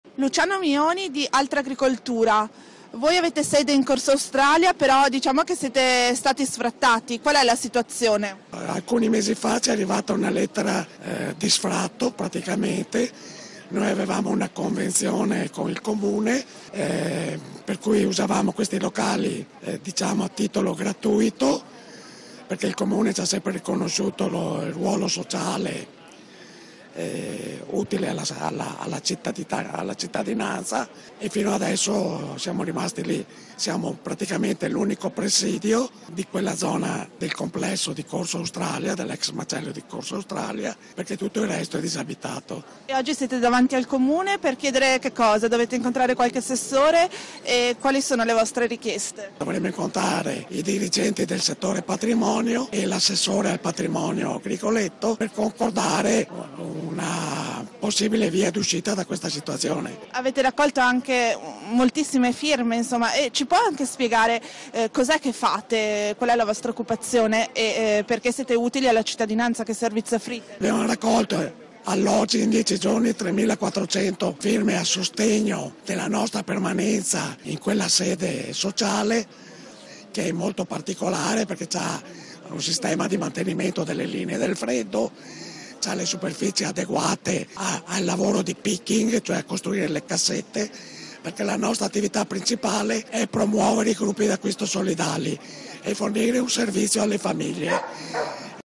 ALTRAGRICOLTURA SFRATTATA: SIT IN DAVANTI IL COMUNE
Nei giorni scorsi l'associazione e gruppo di acquisto Altragricoltura Nord Est ha ricevuto da parte del comune un avviso... [continua a leggere su